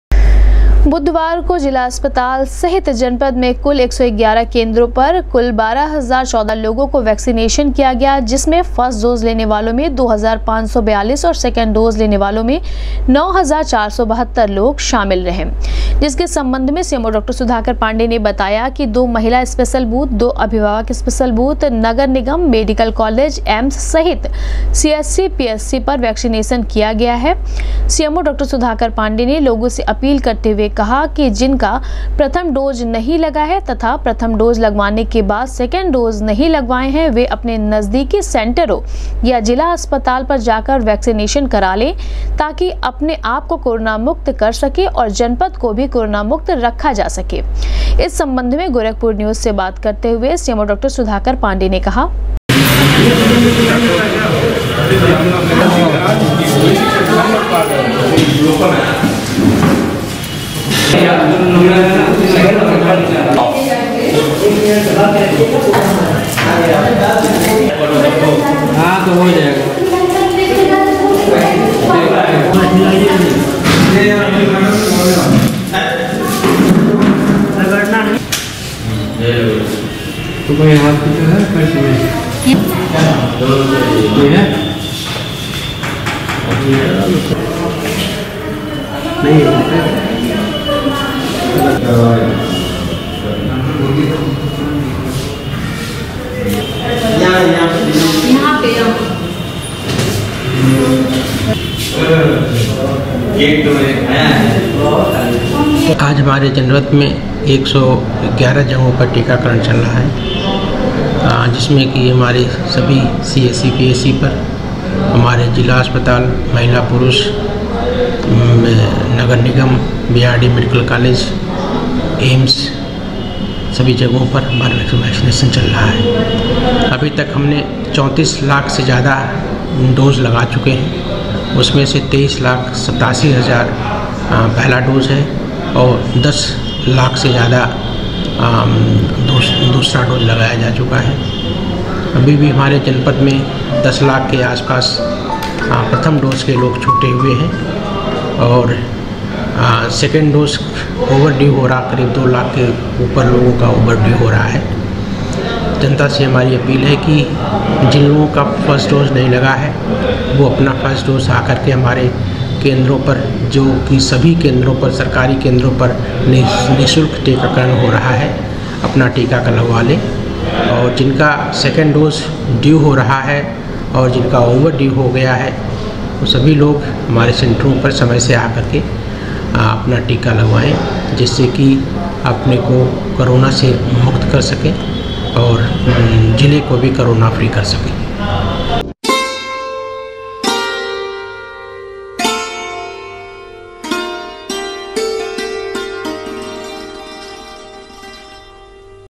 बुधवार को जिला अस्पताल सहित जनपद के 111 केंद्रों पर कुल 12,014 लोगो को किया गया वैक्सीनेशन,फस्र्ट
बुधवार को जिला अस्पताल सहित जनपद के 111 केंद्रों पर कुल 12,014 लोगो को किया गया वैक्सीनेशन,फस्र्ट डोज लेने वालों में 2,542 और सेकेंड डोज लेने वालों में 9,472 लोग रहे शामिल,सीएमओ ने कहा,अवश्य लगवाये टीका जीत का